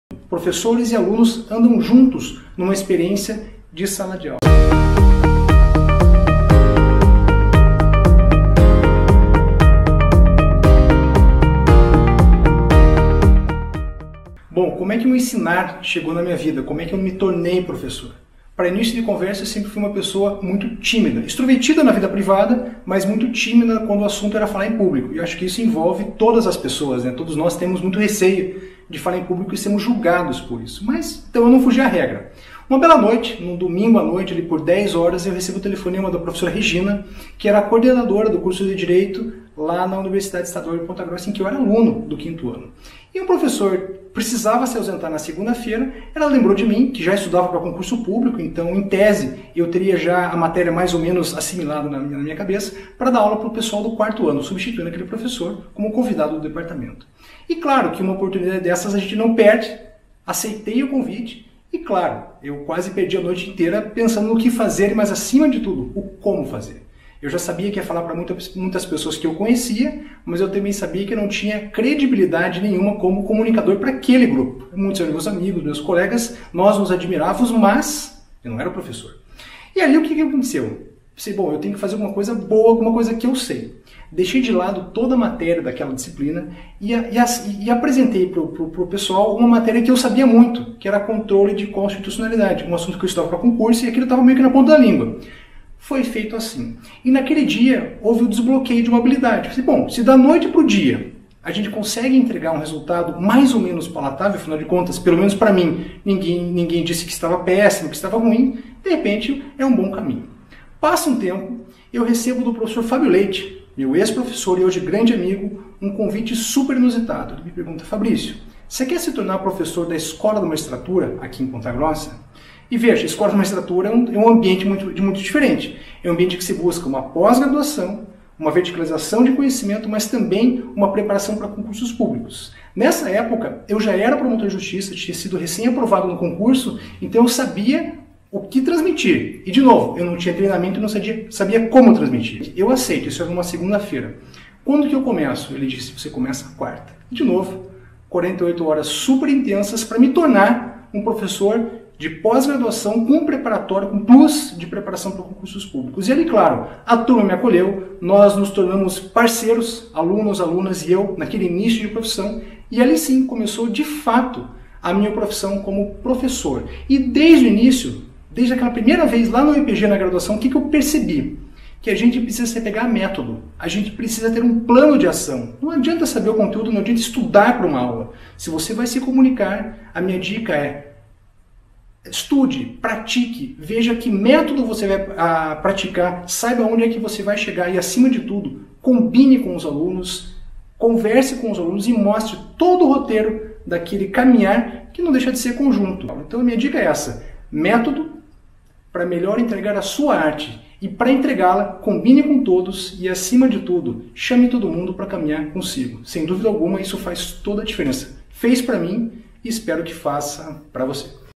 CAMINHAR COM O ALUNO - UM PERCURSO MAIS PROVEITOSO
Convidar o aluno a caminhar lado a lado com você pode tornar o trajeto muito mais tranquilo e proveitoso pra todo mundo. Neste vídeo eu trago a minha primeira experiência a frente de uma turma de alunos e esse foi o primeiro método que eu usei nas minha vida como professor. Entrar num acordo com a turma sobre os caminhos que serão percorridos é a dica que eu quero dar hoje.